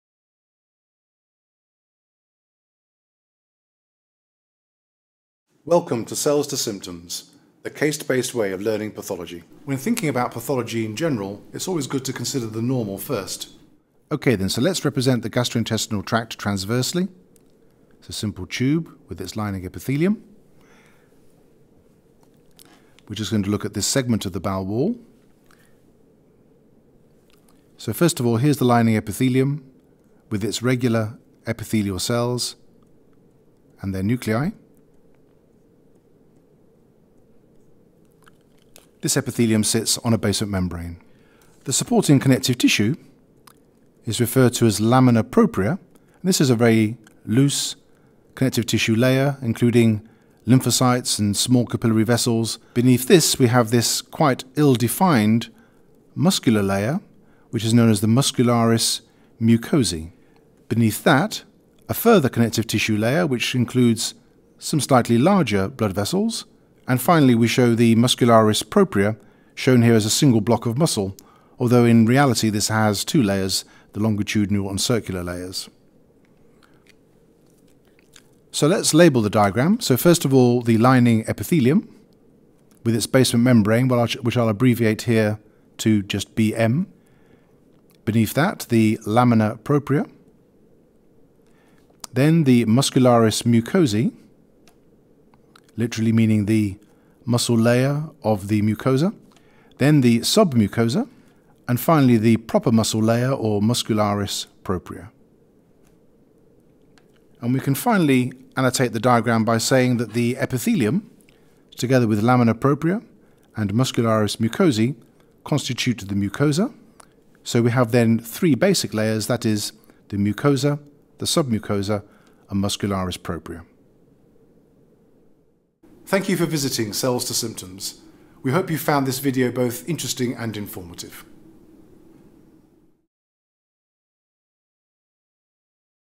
Layers of the Gastrointestinal Tract - Cells to Symptoms
Taken from the Cells to Symptoms clinical case 'Tired all the time'